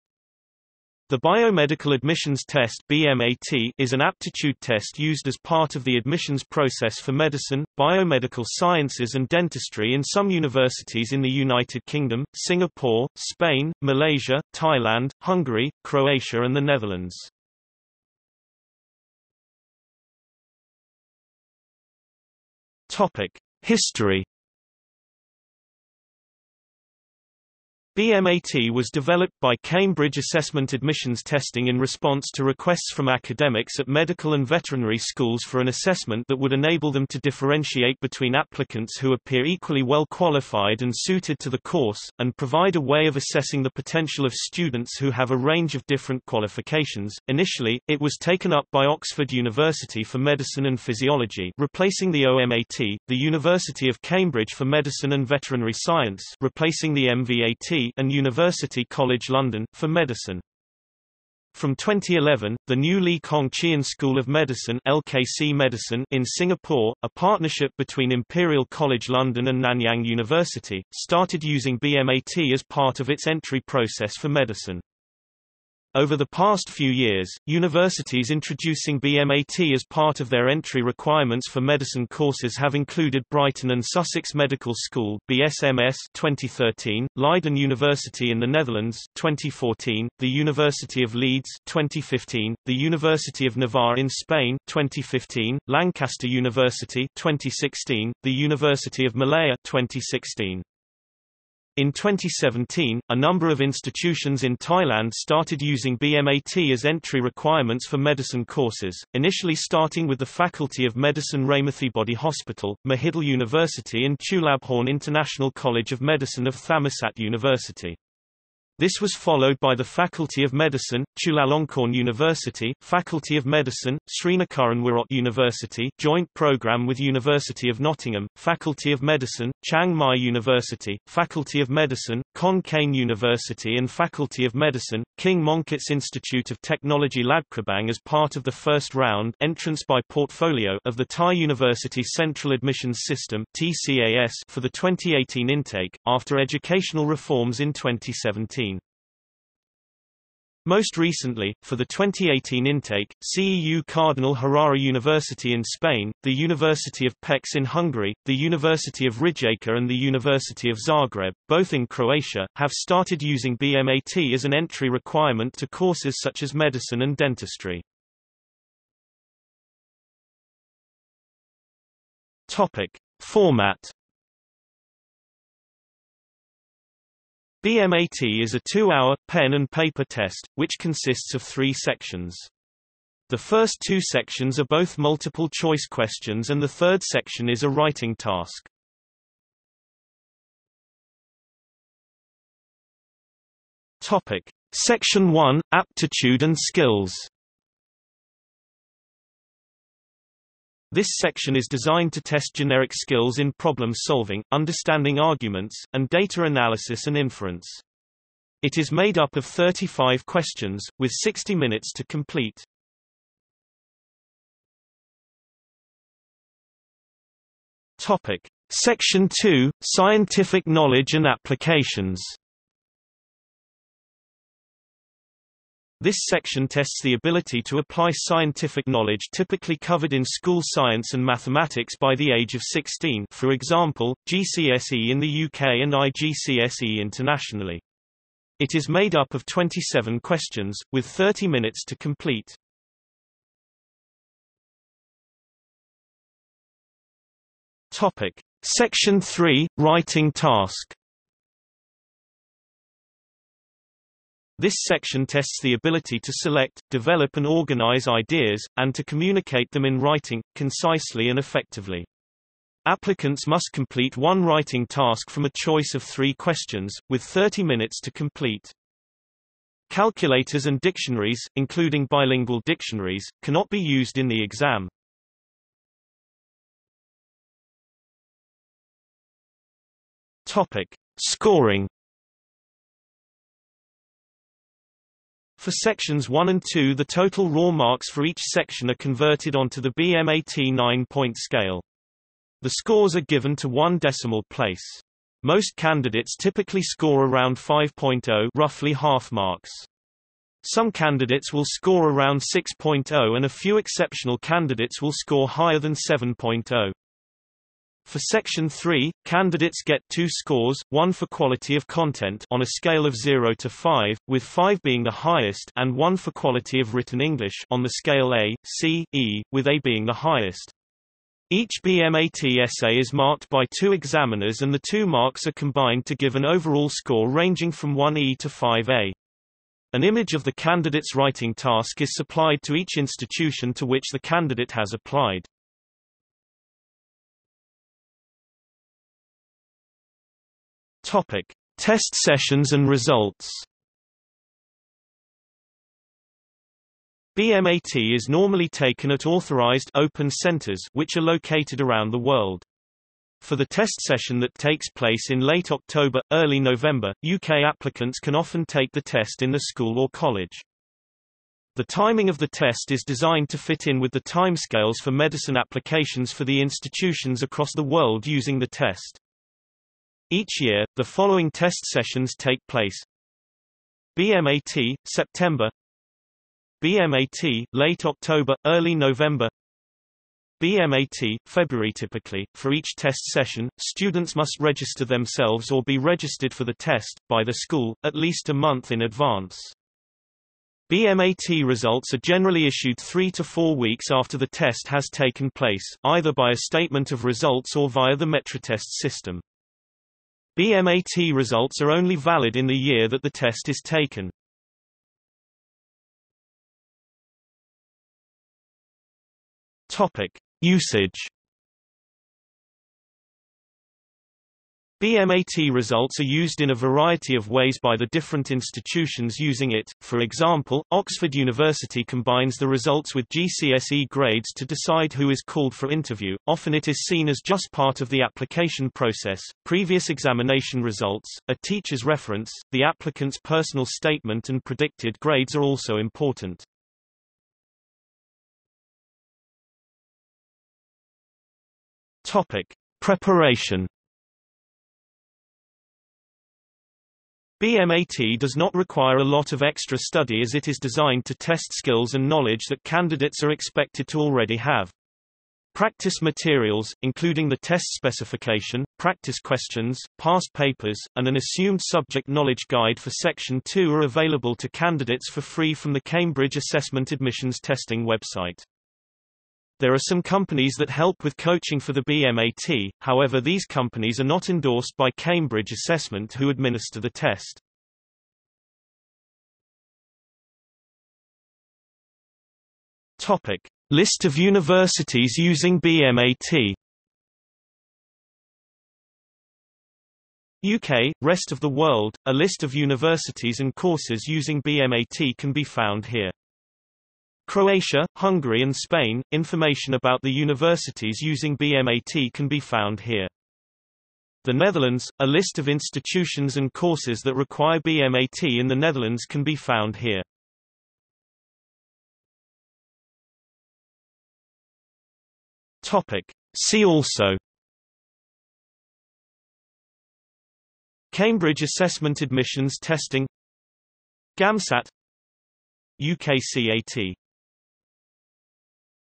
BioMedical Admissions Test | Wikipedia audio article
00:00:19 1 History
00:02:48 2 Format
00:03:10 2.1 Section 1: Aptitude and Skills
00:03:34 2.2 Section 2: Scientific Knowledge and Applications
00:04:04 2.3 Section 3: Writing Task
00:04:36 3 Scoring
00:05:37 4 Test sessions and results
00:06:46 5 Usage
00:07:18 6 Preparation
00:07:59 7 List of universities using BMAT
00:08:32 8 See also

- Socrates

SUMMARY
The BioMedical Admissions Test (BMAT) is an aptitude test used as part of the admissions process for Medicine, Biomedical Sciences and Dentistry in some universities in the United Kingdom, Singapore, Spain, Malaysia, Thailand, Hungary, Croatia and the Netherlands.